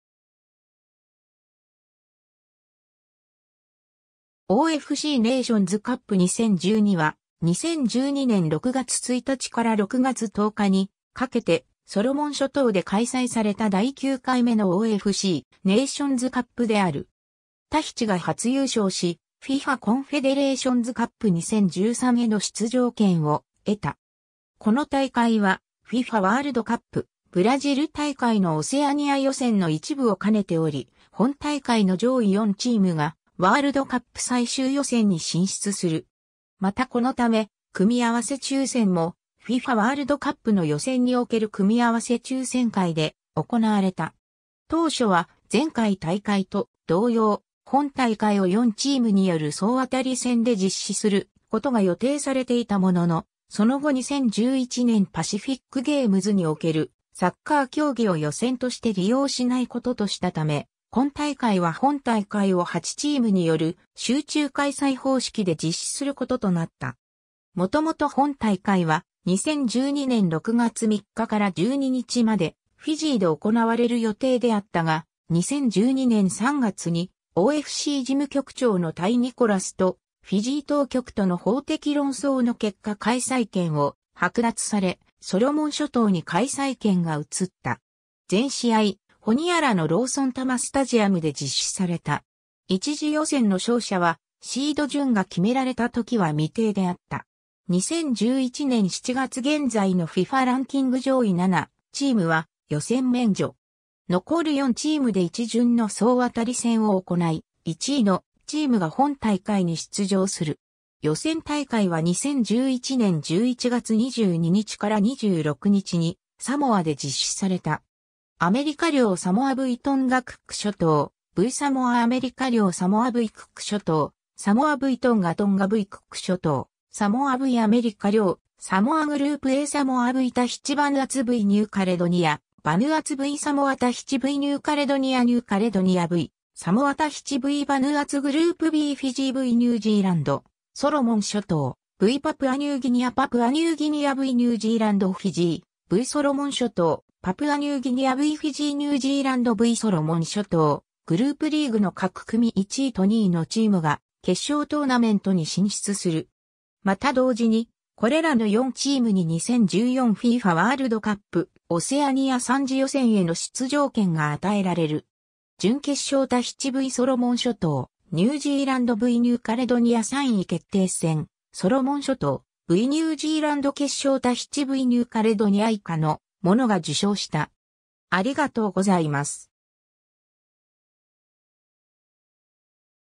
OFCネイションズカップ2012
2014_FIFAワールドカップ・オセアニア予選
OFCネイションズカップ2012（英: 2012 OFC Nations Cup）は、2012年6月1日から6月10日にかけて、ソロモン諸島で開催された第9回目のOFCネイションズカップである。
 タヒチが初優勝し、FIFAコンフェデレーションズカップ2013への出場権を得た。
 この大会はFIFAワールドカップ・ブラジル大会のオセアニア予選の一部を兼ねており、本大会の上位4チームがワールドカップ最終予選に進出する。
またこのため、組み合わせ抽選もFIFAワールドカップの予選における組み合わせ抽選会で行われた（2011年7月30日にブラジル・リオデジャネイロで実施）。
 当初は前回（2008年）大会と同様、本大会を4チームによる総当たり戦で実施することが予定されていたものの、その後2011年パシフィックゲームズにおけるサッカー競技を予選として利用しないこととしたため（前回大会は予選として利用していた）、今大会は本大会を8チームによる集中開催方式で実施することとなった。
 元々本大会は、2012年6月3日から12日までフィジーで行われる予定であったが、2012年3月にOFC事務局長のタイ・ニコラスとフィジー当局との法的論争の結果開催権を剥奪され、ソロモン諸島に開催権が移った。
 全試合、ホニアラのローソン・タマ・スタジアムで実施された。
 † 1次予選の勝者はシード順が決められた時は未定であった。
 2011年7月現在のFIFAランキング上位7チームは予選免除。
残る4チームで1順の総当たり戦を行い、1位のチームが本大会に出場する。
 予選大会は2011年11月22日から26日にサモアで実施された。
また同時に、これらの4チームに2014 FIFAワールドカップ・オセアニア3次予選への出場権が与えられる（ワールドカップ3次予選の試合は別途実施される）。
 準決勝 タヒチ v ソロモン諸島 ニュージーランド v ニューカレドニア 3位決定戦 ソロモン諸島 v ニュージーランド 決勝 タヒチ v ニューカレドニア 以下の者が受賞した。